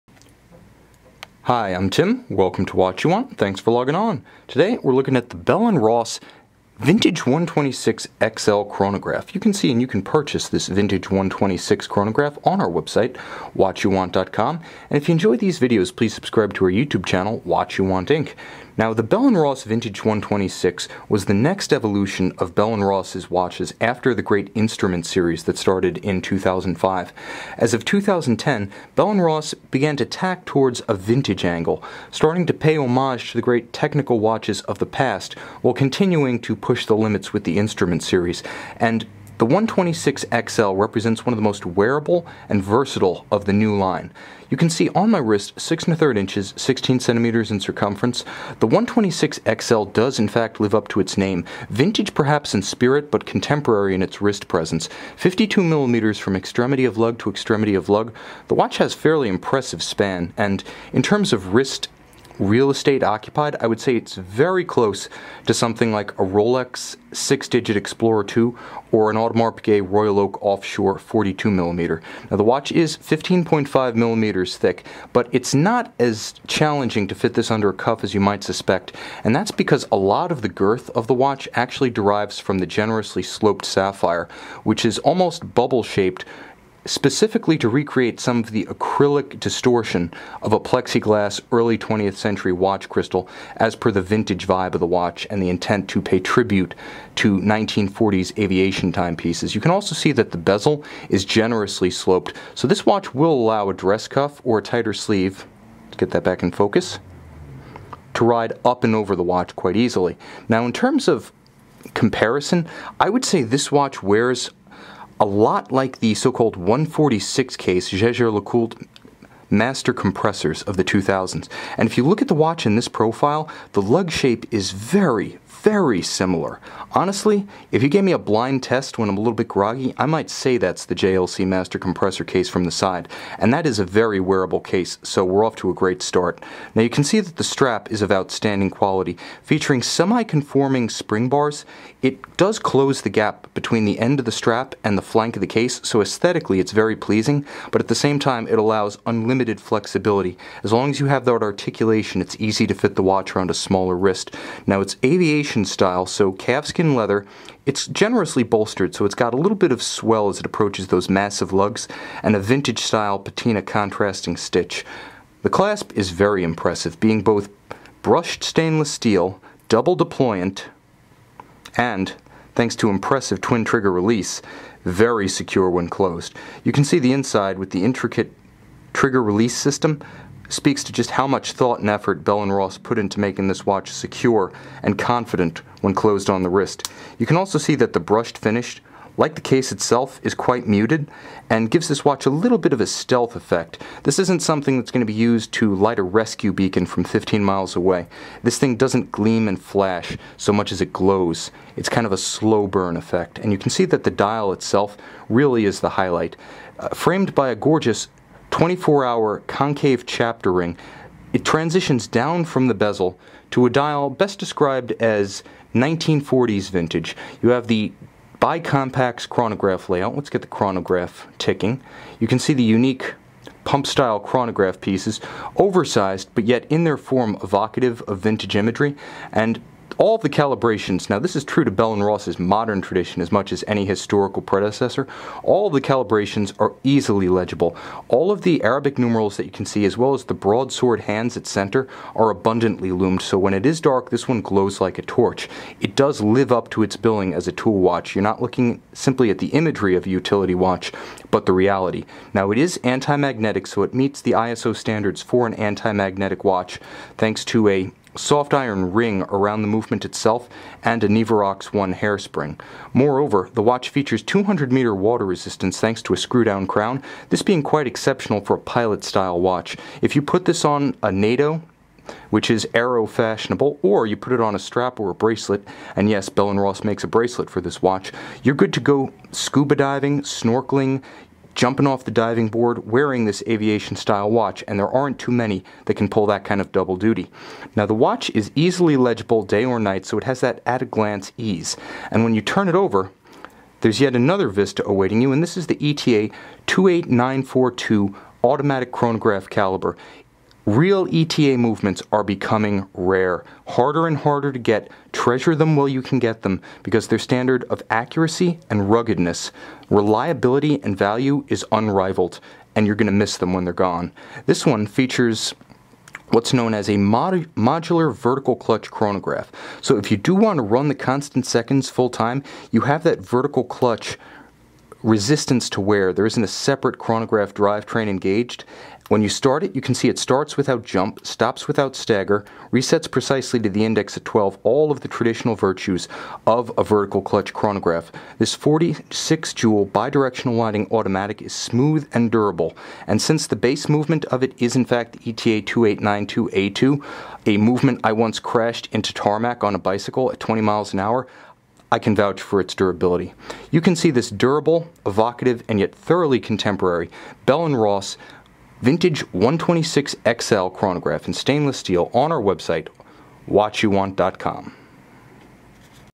Bell & Ross Vintage 126 XL Chronograph Luxury Watch Review
See this 42.5mm stainless steel Bell & Ross Vintage 126 XL Chronograph in high-resolution images on our